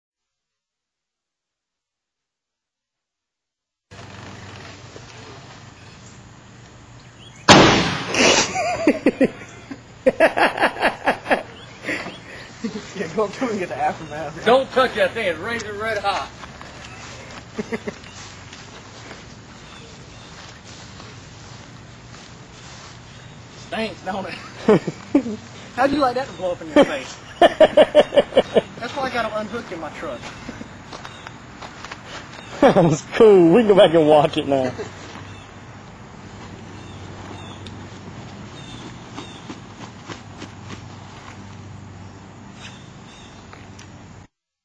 air bag pop
some rednecks set off an airbag in the front yard